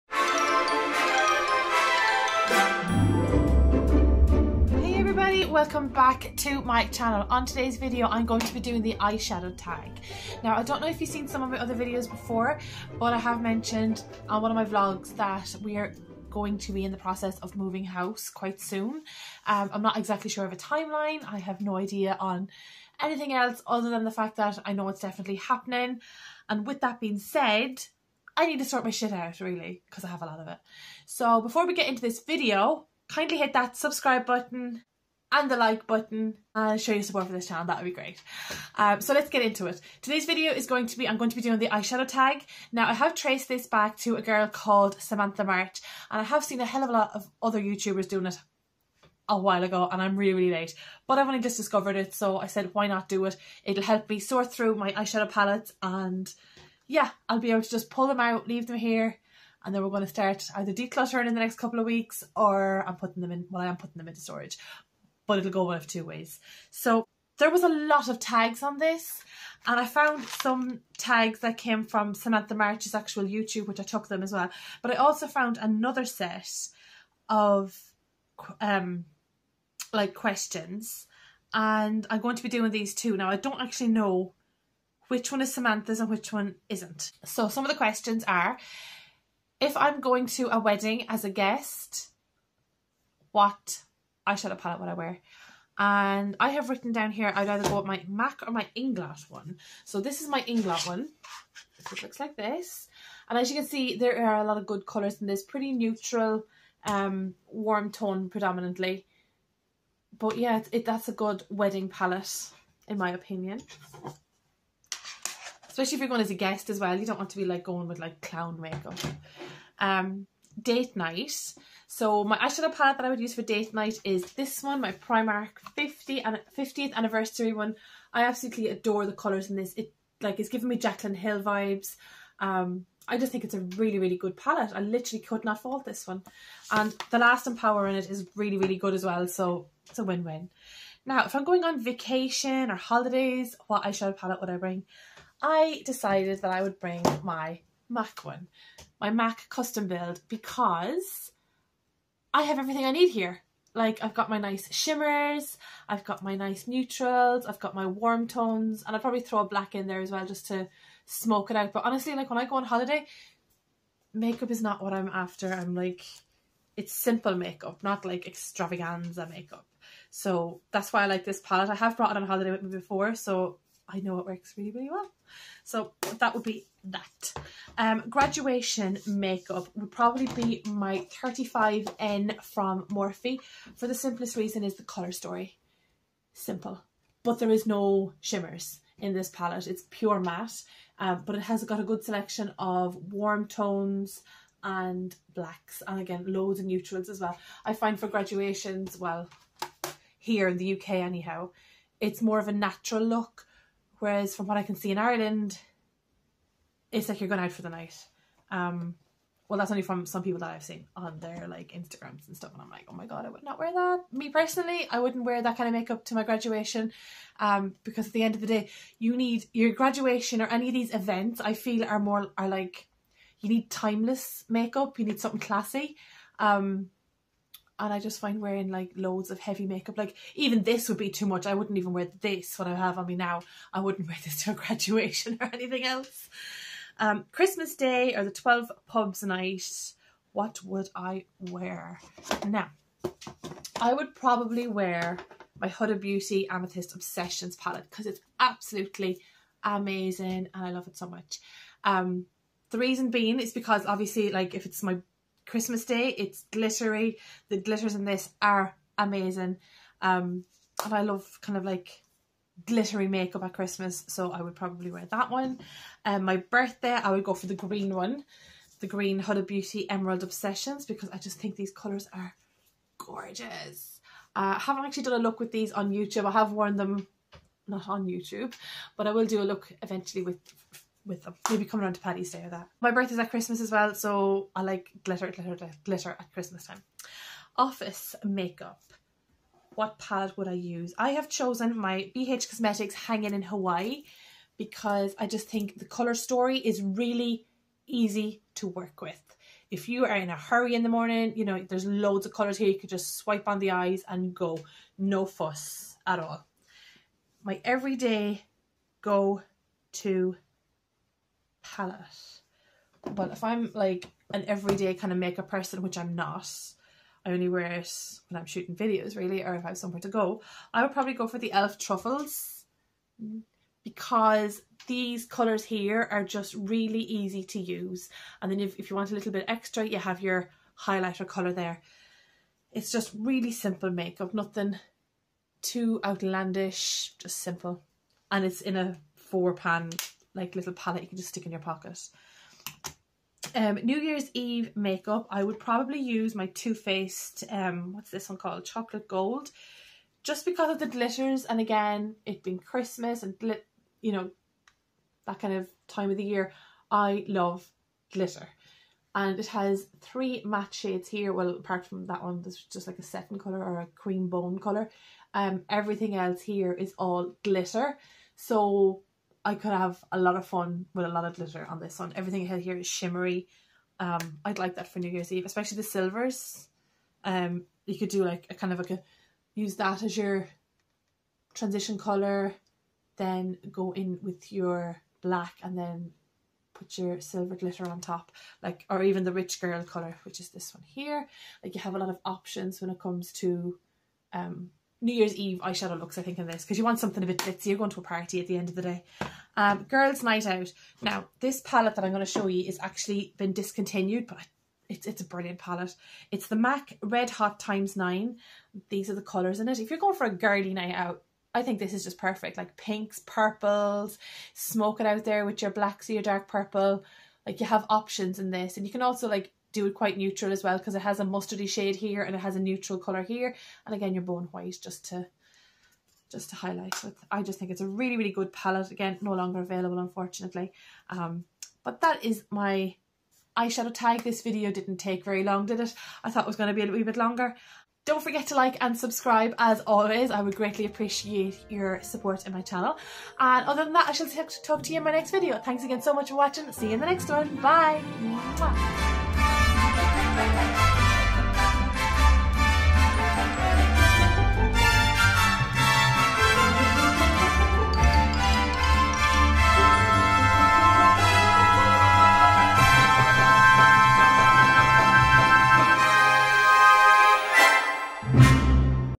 Eyeshadow palette tag 2022 || revisiting eyeshadow palette tag
Hey guys ,
I decided this year I’m going to do more, so I’ve done my first tag! I traced it back to Samantha March. So credit for this idea goes to her!
I really enjoyed looking through my pallets to find them for the video.
Xoxo💕